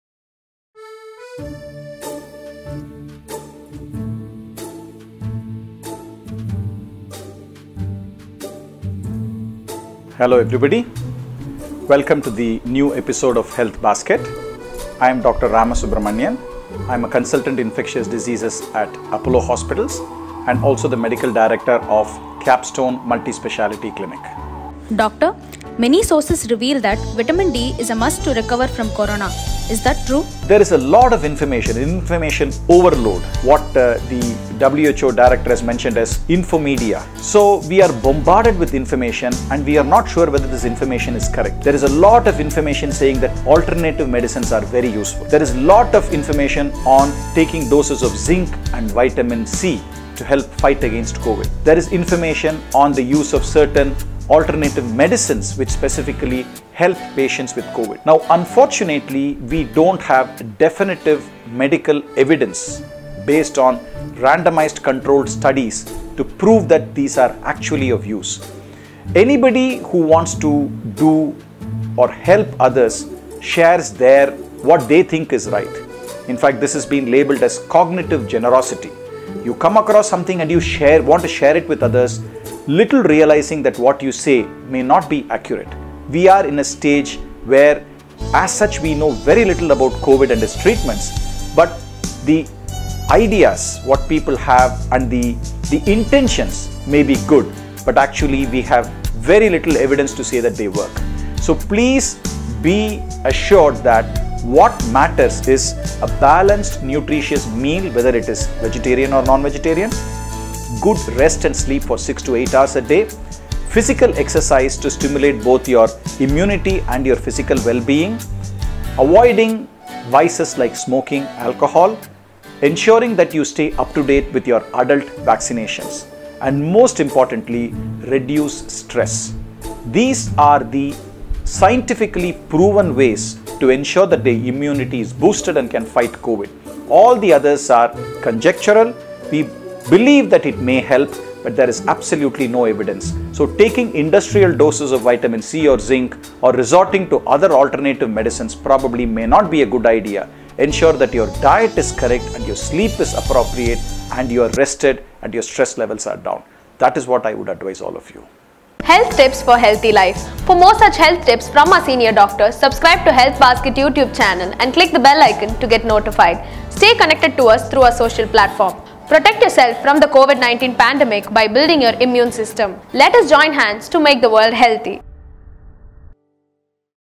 Cure for corona? COVID 19 and Vitamin D | Dr. V. Ramasubramanian
Delivered by Dr. V. Ramasubramanian

MBBS, MRCP, Diploma in Tropical medicine and Hygiene
Dept. of infectious disease, Apollo Hospitals, Capstone clinic